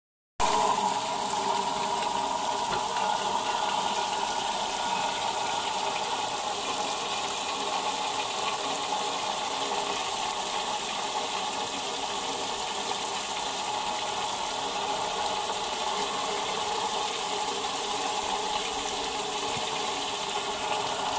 Egylépcsős oszlopos pálinkafőző 2. - CIP üstmosó fej első próbája
Pálinkafőző CIP üstmosó fej első próbája hálózati víznyomással, "fedezékből".